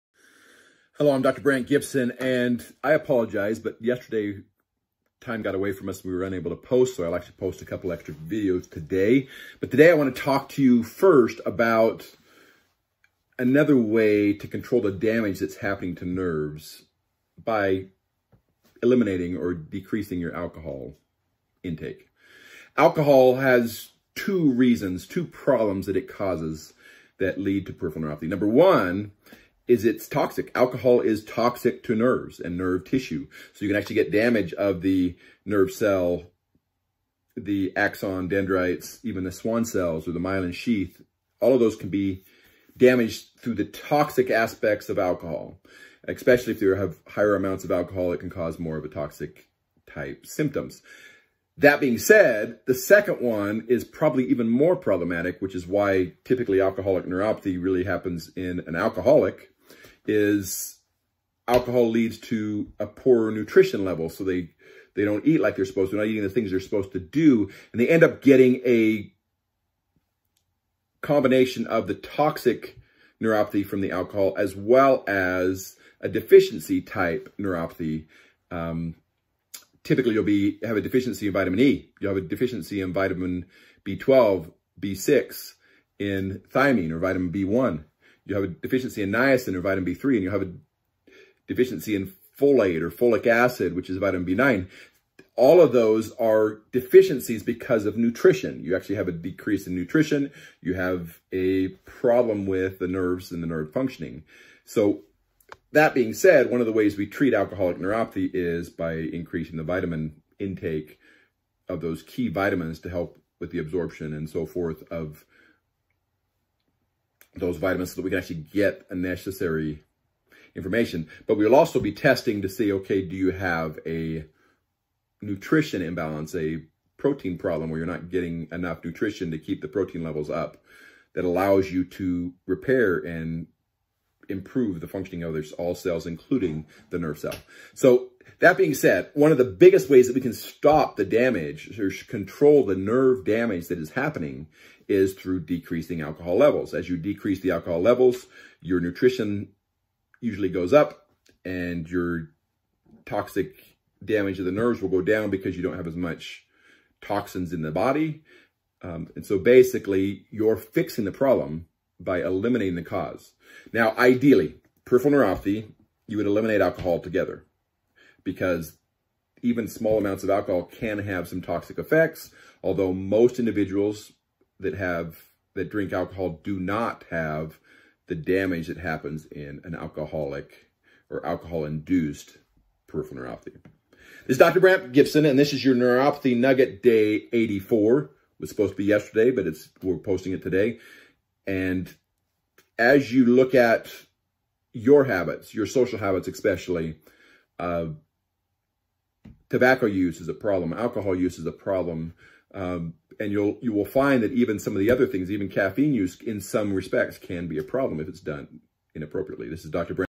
Control Neuropathy - Limit Alcohol
Neuropathy Nugget Day 84 - Today we discuss Alcohol induce Neuropathy.  Dr Brandt R Gibson explains how alcohol can damage nerves and why it should be limited.